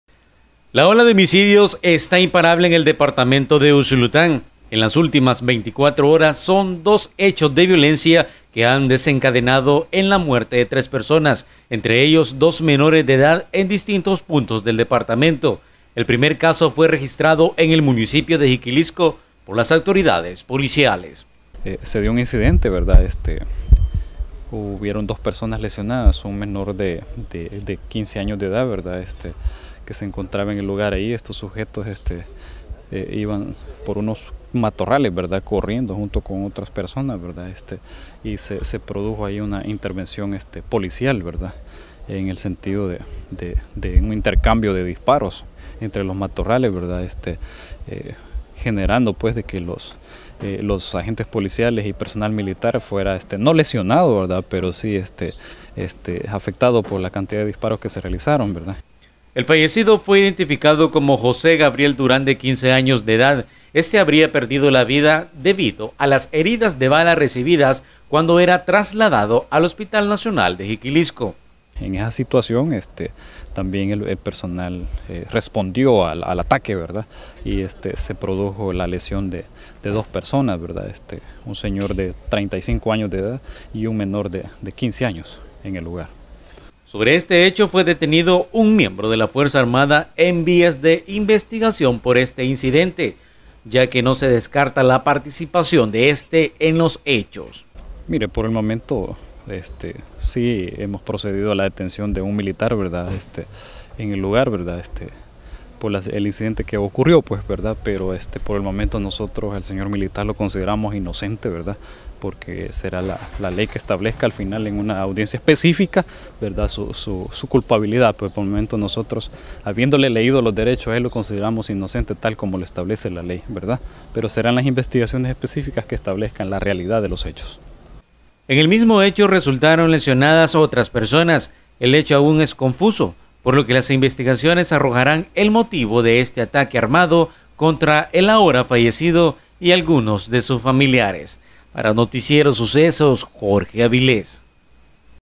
Menor de 15 años fue asesinado en Jiquilisco
La ola de homicidios esta imparable en el departamento de Usulután, en las últimas 24 horas son 2 hechos de violencia que han desencadenado en la muerte de tres personas entre ellos dos menores de edad en distintos puntos del departamento, el primer caso fue registrado en el municipio de Jiquilisco por las autoridades policiales.